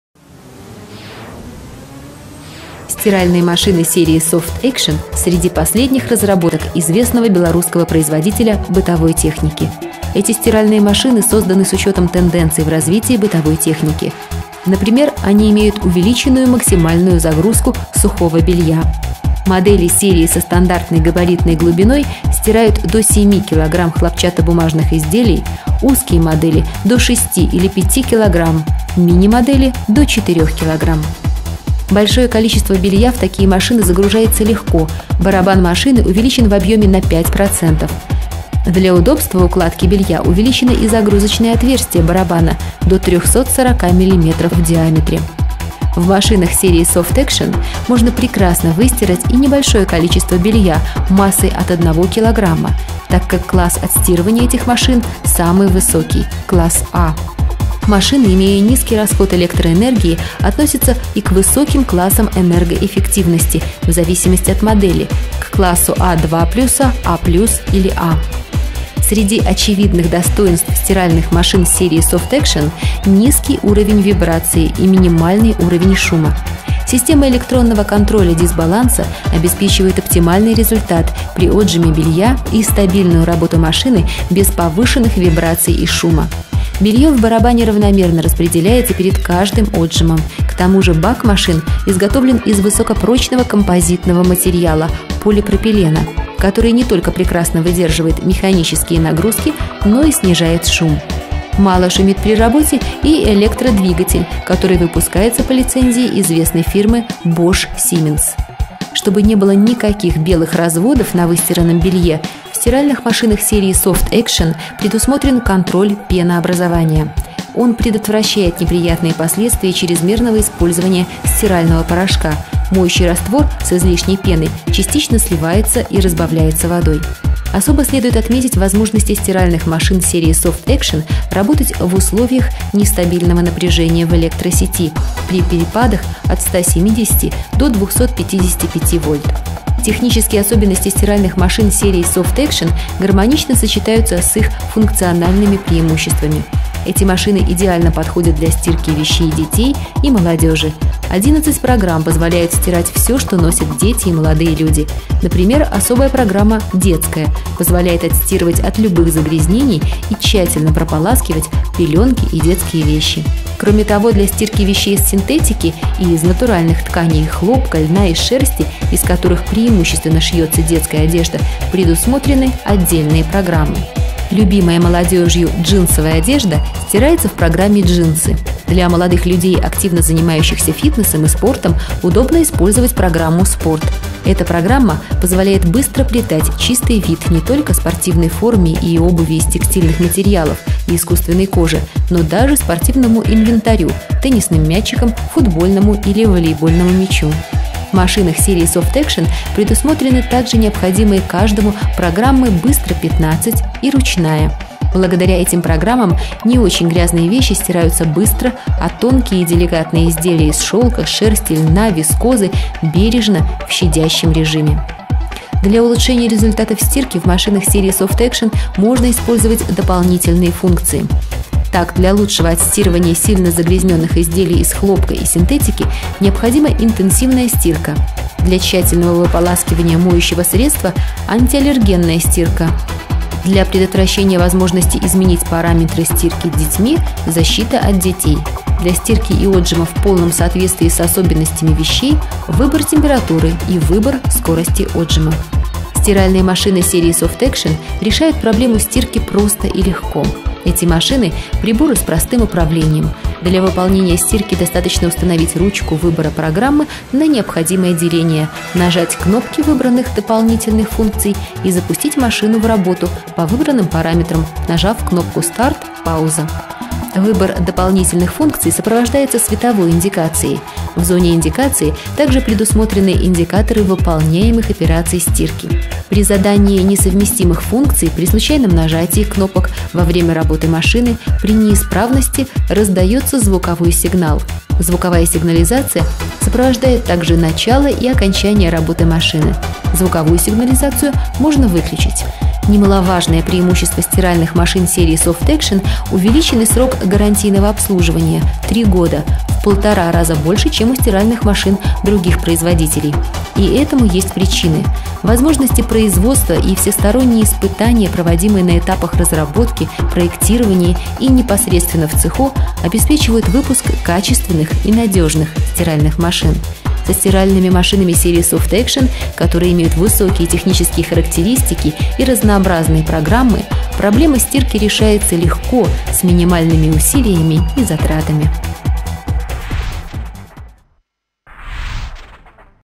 Стиральные машины ATLANT 9 серии SOFT ACTION
АТЛАНТ. Шесть моделей стиральных машин из новой серии SOFT ACTION с высокими техническими характеристиками и разнообразными программами освоены в производстве компанией «ATLANT».
Стиральные машины ATLANT серии SOFT ACTION имеют увеличенную загрузку и высокие классы энергоэффективности — А+++, А++ или А+. 11 программ стирки идеально подходят для вещей для детей и молодежи. Простое управление, низкий уровень вибрации и минимальный уровень шума, увеличенный до 3 лет гарантийный срок службы — среди преимуществ стиральных машин серии SOFT ACTION.